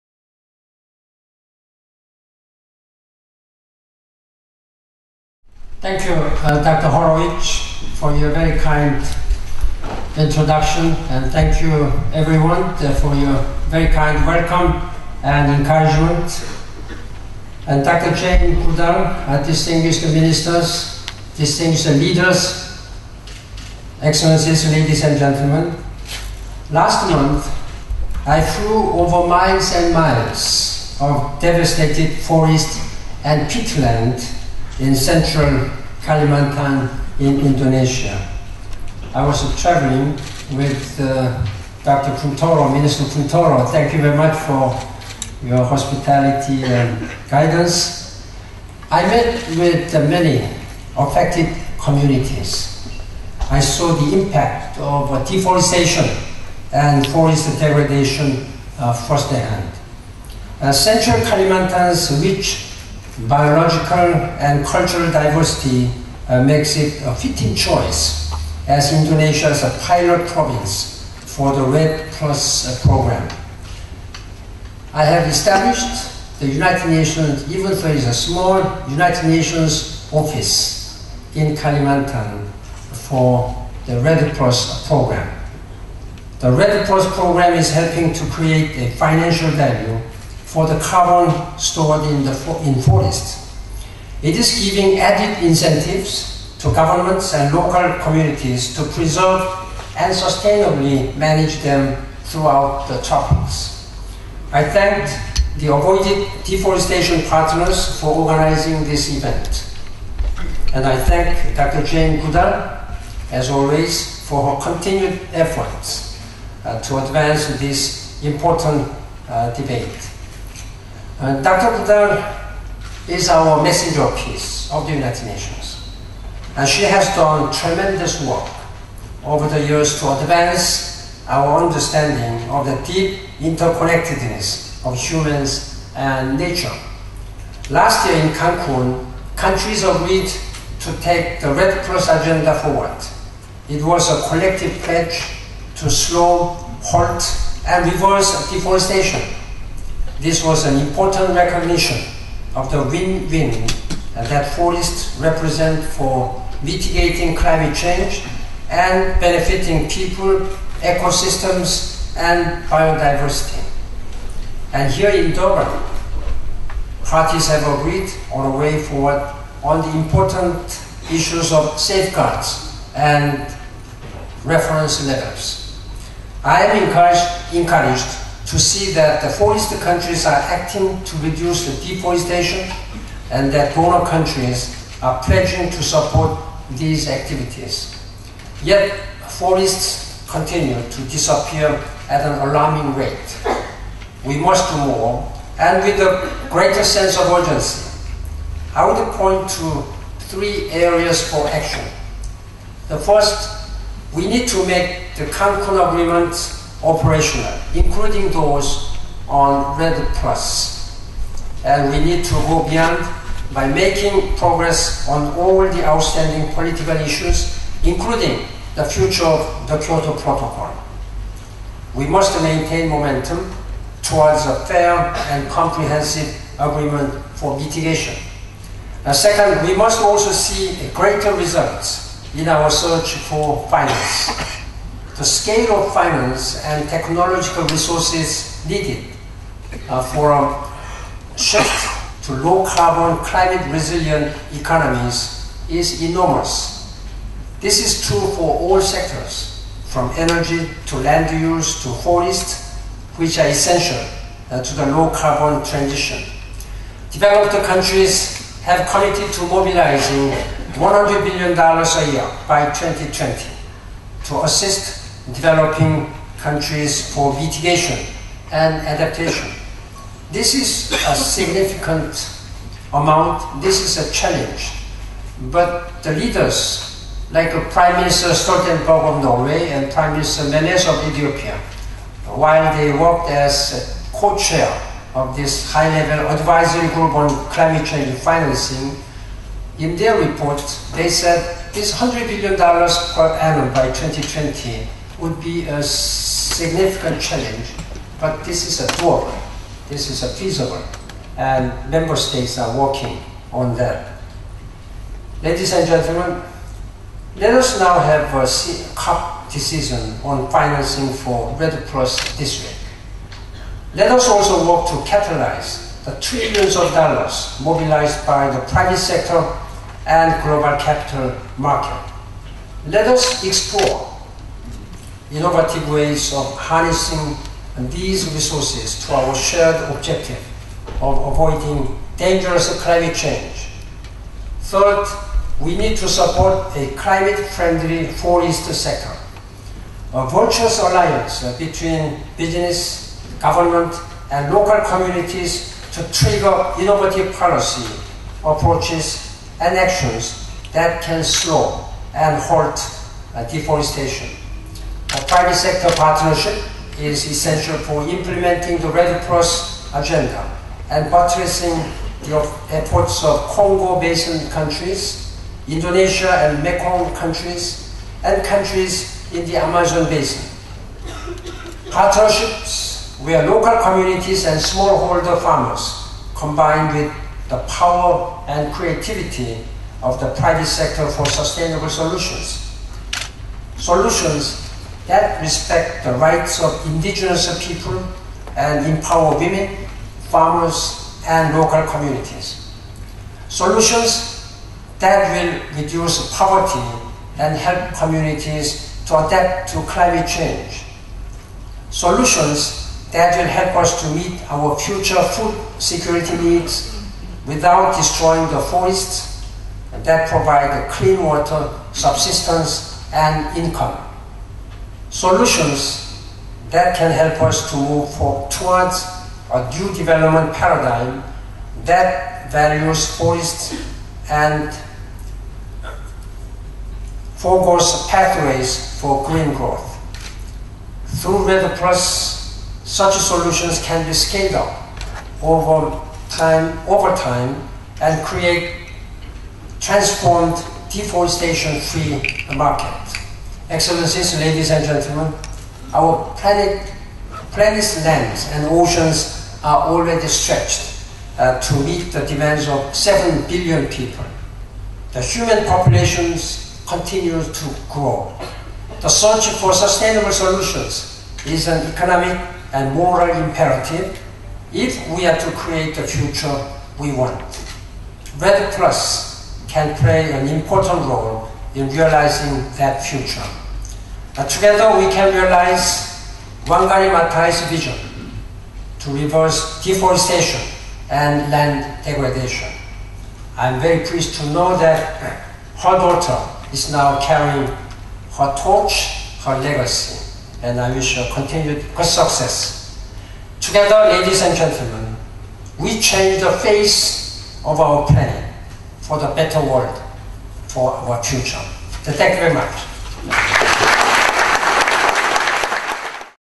Ban Ki-moon on REDD and the need to stop deforestation
UN Secretary General Ban Ki-moon speaks about REDD and halting deforestation at the Avoided Deforestation Partners' high-level REDD+ event on the sidelines of the UNFCCC COP17 in Durban, South Africa. He highlights the need for urgent action and identifies three areas for progress.